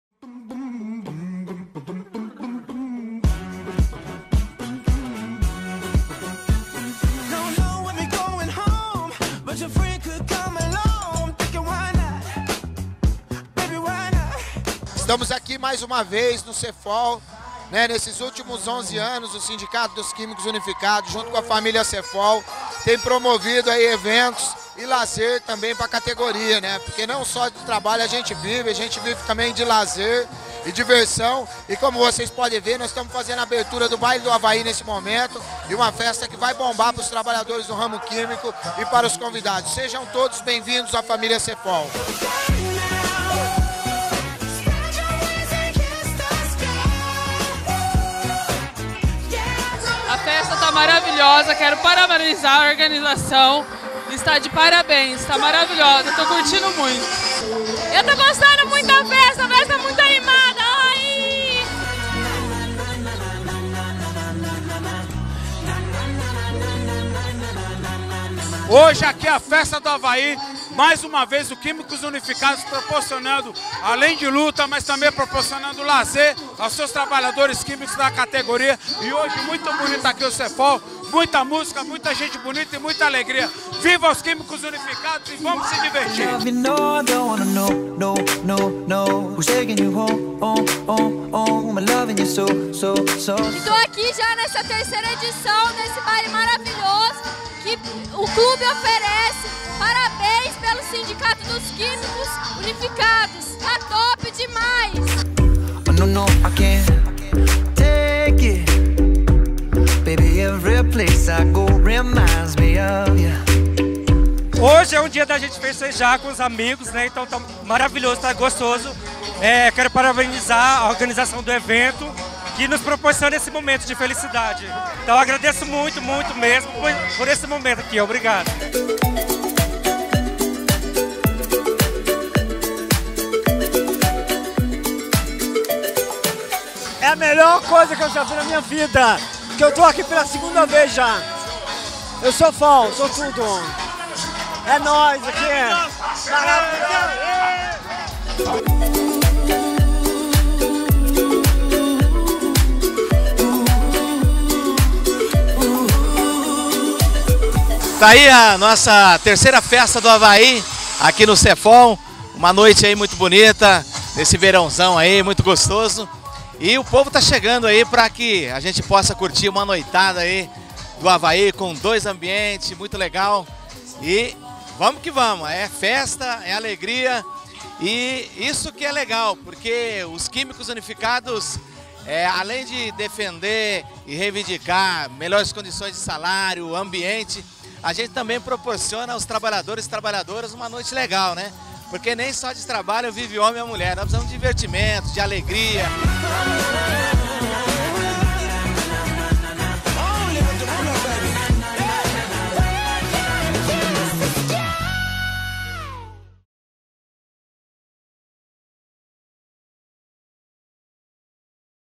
CEFOL - BAILE DO HAWAII 2016
Um pouquinho do que foi o Baile do Hawaii neste ano... você curtiu? Cerca de 1.500 pessoas estiveram conosco nesta festa linda!